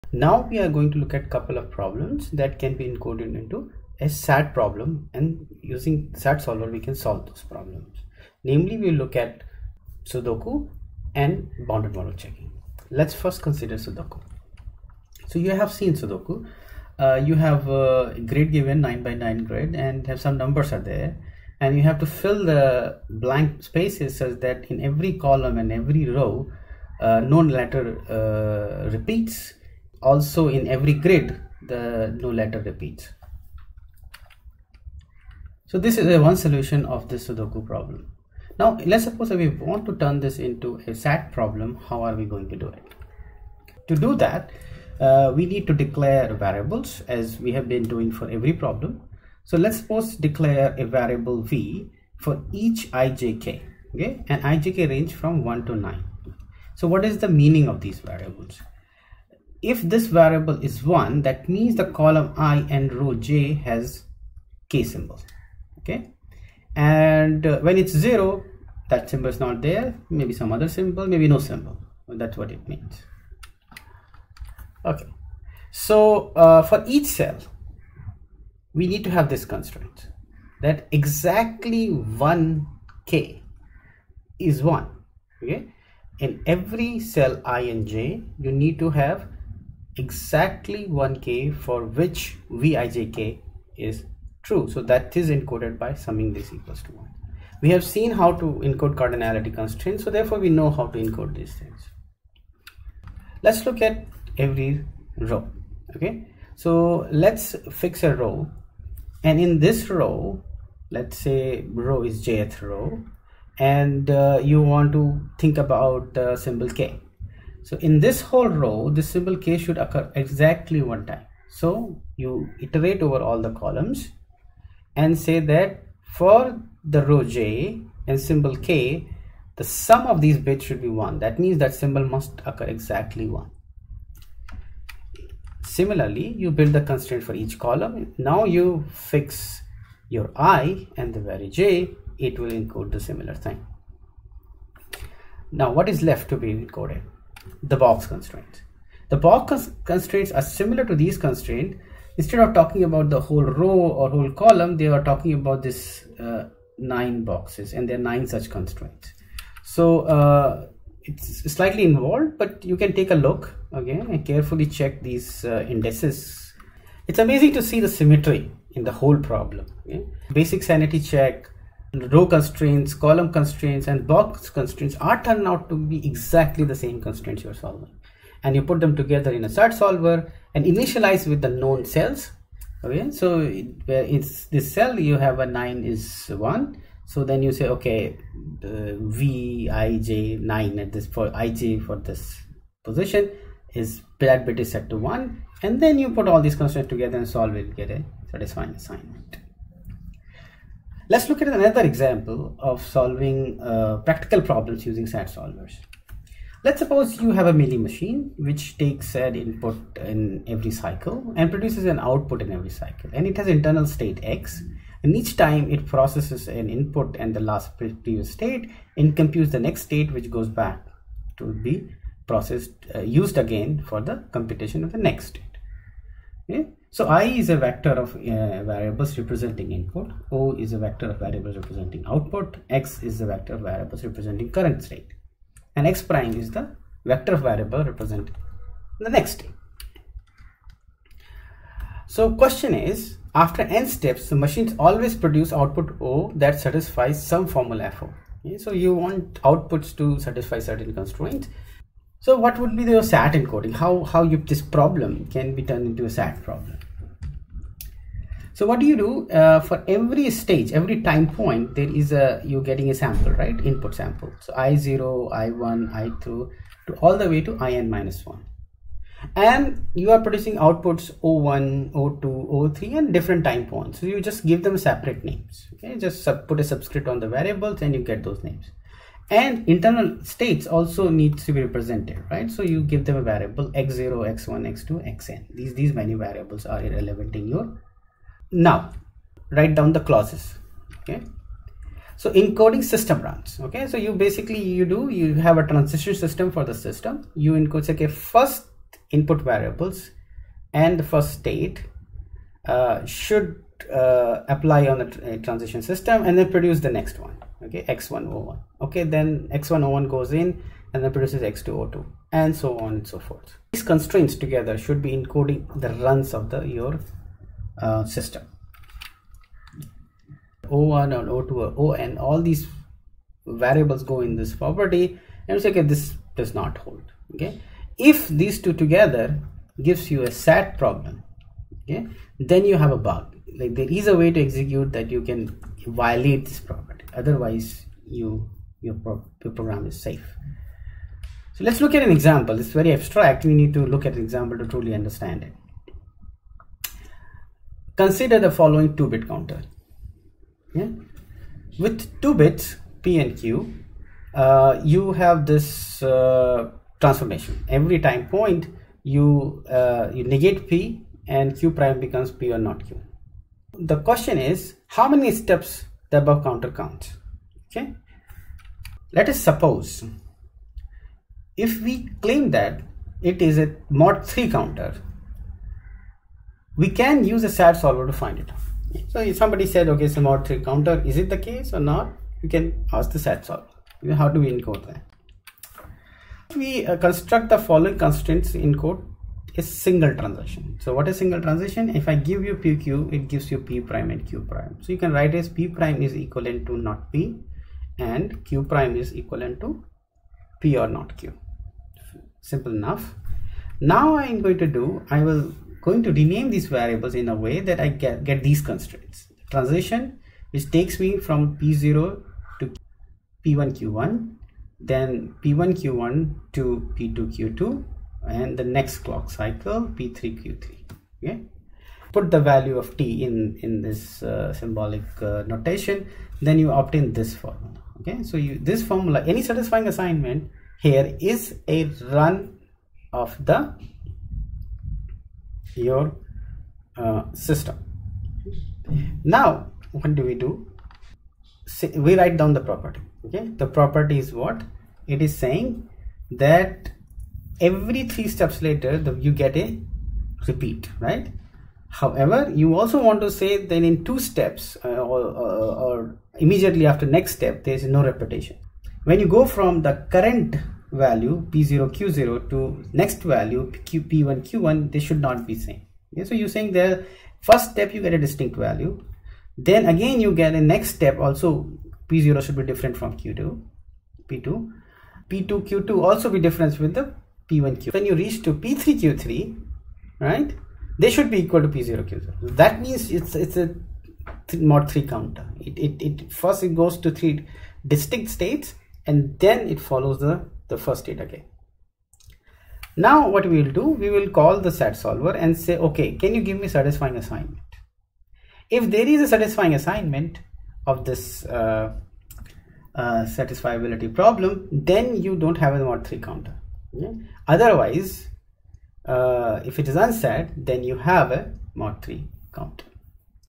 Lecture 11-3 Encoding Sudoku and bounded model checking (Lecture 8 in CS433)
We see encoding of two practical problem.